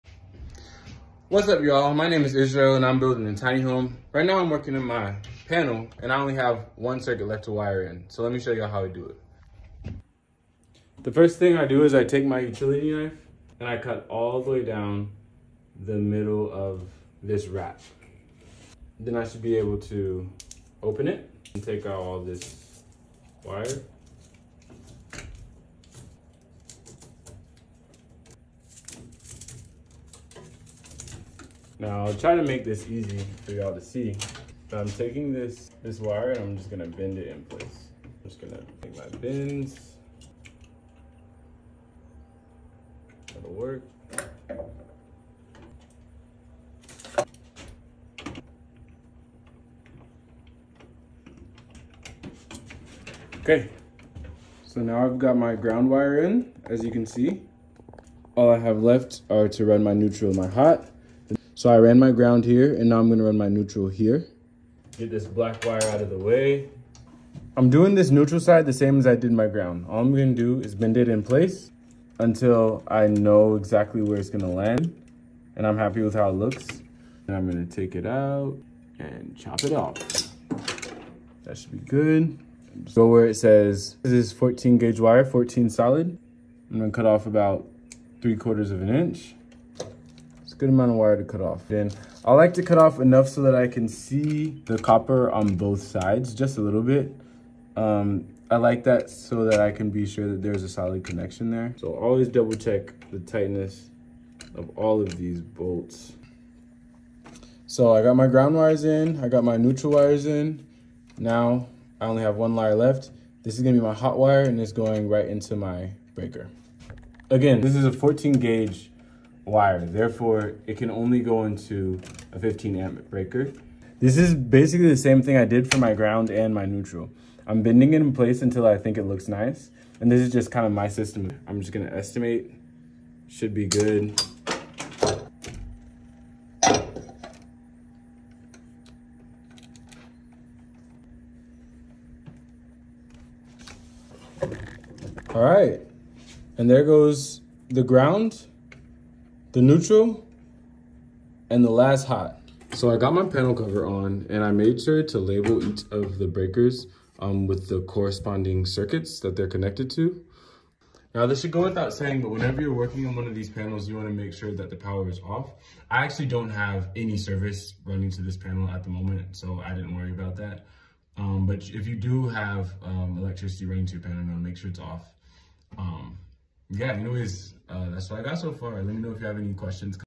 Tiny Home Build: Installing Breakers and Wiring a New Circuit
Here, I show how I wired a new circuit into a breaker panel for my in construction tin home. I go step-by-step through how to route the wire, strip conductors, and connect them properly to the breaker, neutral, and ground bars.

Safety Note: Always turn off main power before working inside a panel, follow local electrical codes, and consult a licensed electrician if you’re unsure.

Questions comments and concerns always encouraged

stay blessed